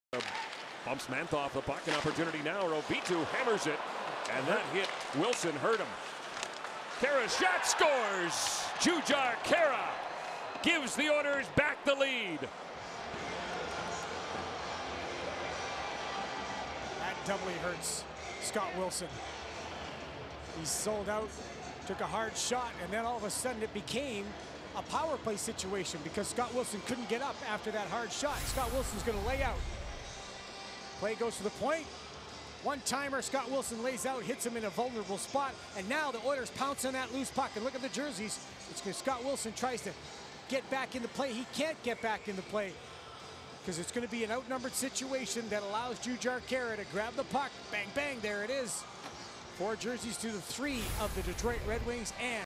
Jujhar Khaira Goal vs DET 11-22-17
Jujhar Khaira 2 goal of 2017-18 nhl season. even goal assisted by Ryan Strome and Yohann Auvitu